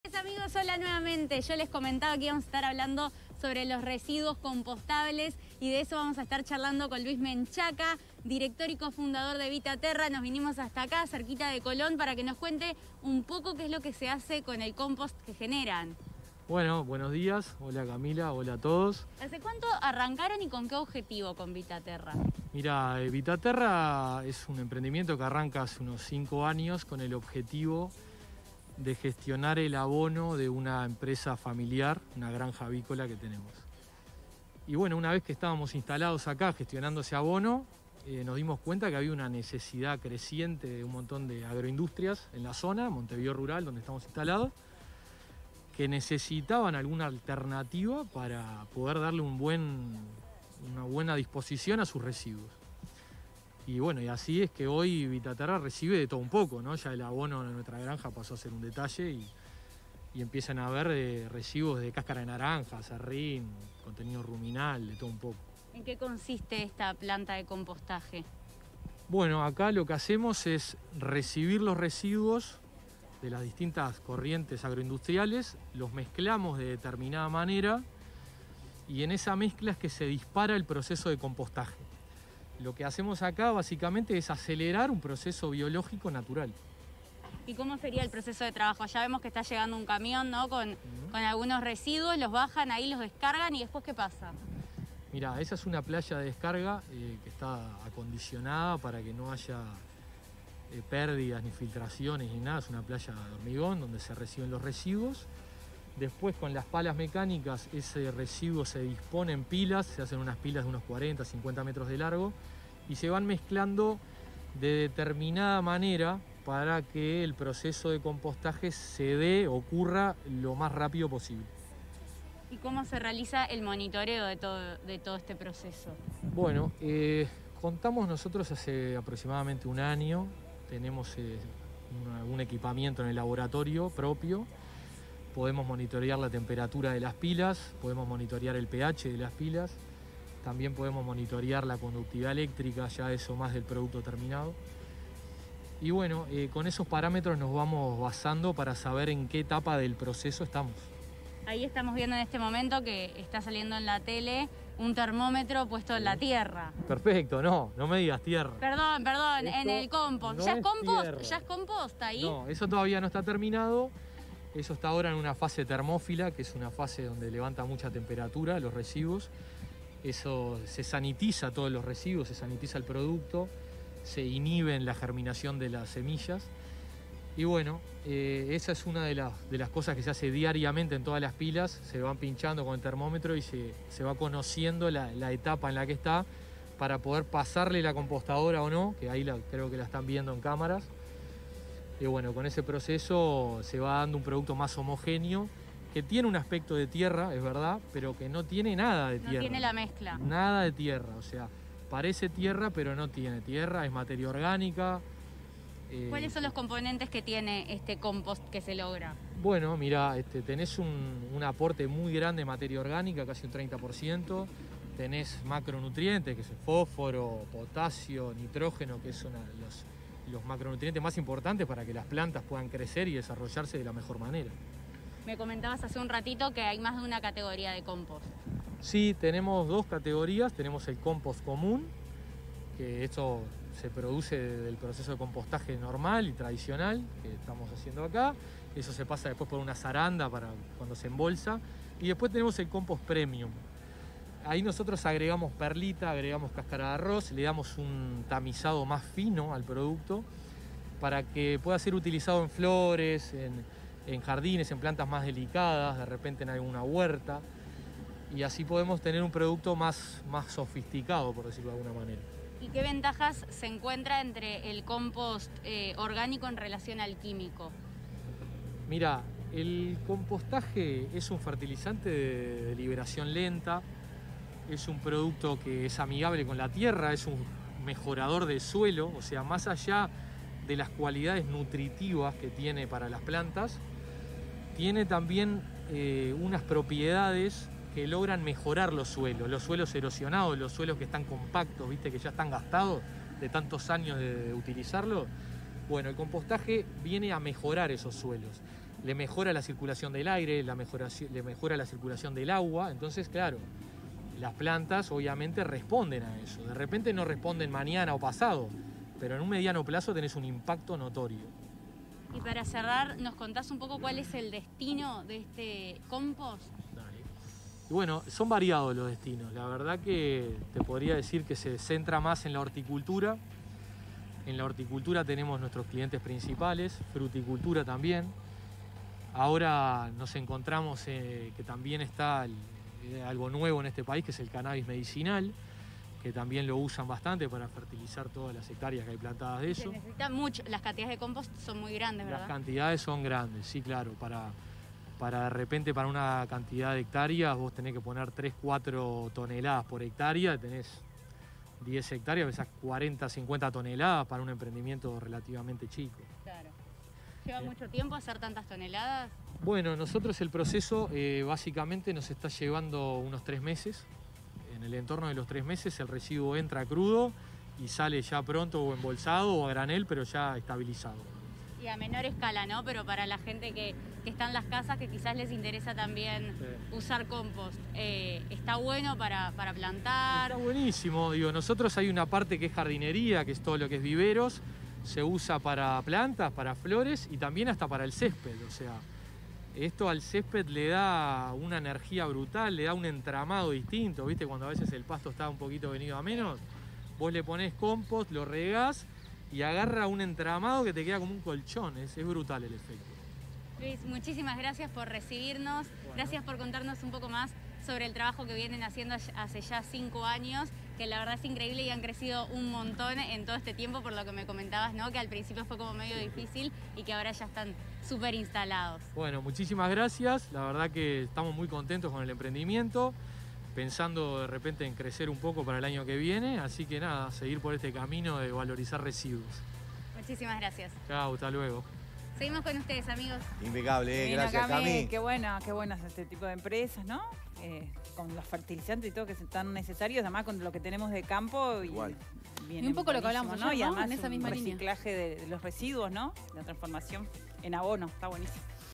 La planta de compostaje de Vitaterra
La planta de compostaje recibe residuos orgánicos de agroindustriales de la zona.